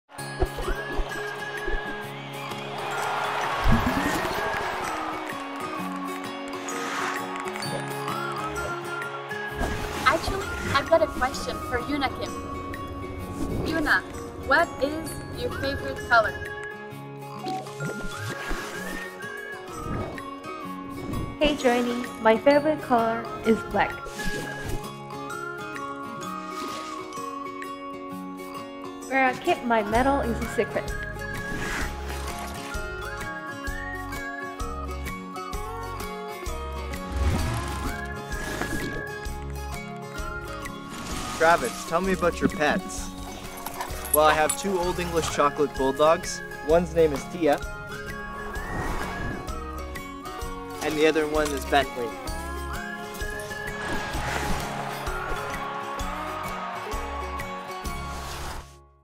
New Version of Olympic Athletes' Hub App Available
Check out Olympians Joannie Rochette, Yuna Kim, and Travis Gerrits using the new version of the Olympic Athletes' Hub app.